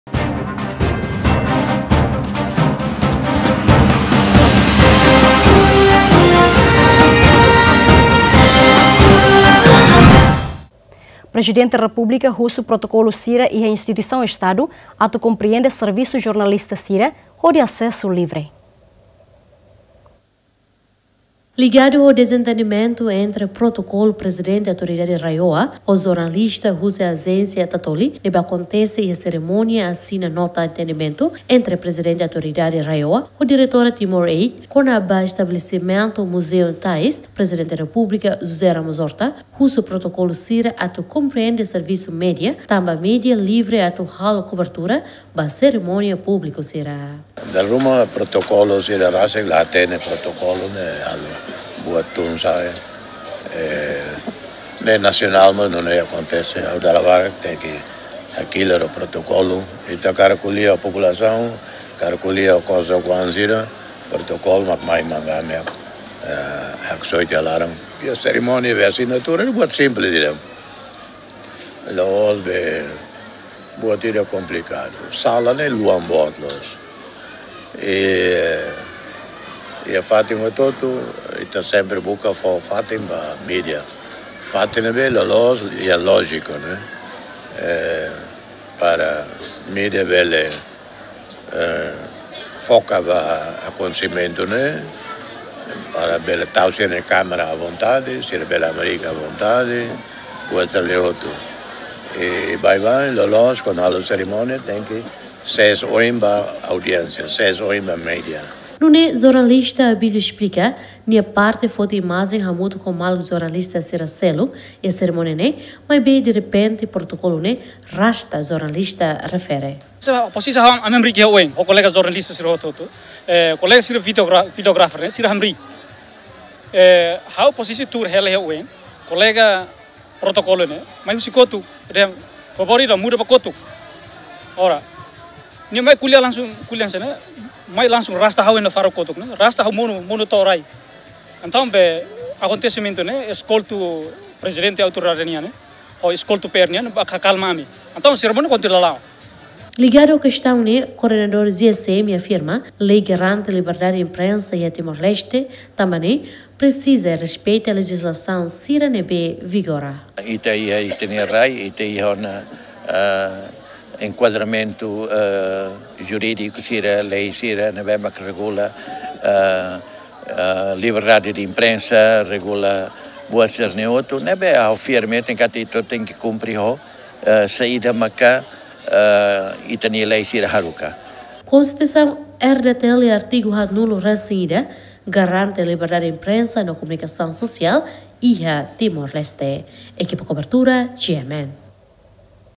PR husu protokolu Instituisaun Estadu sira atu kompriende servisu jornalista sira hodi asesu livre
Prezidenti Republika husu protokolu sira iha Instituisaun Estadu atu kompriende servisu jornalista sira hodi asesu livre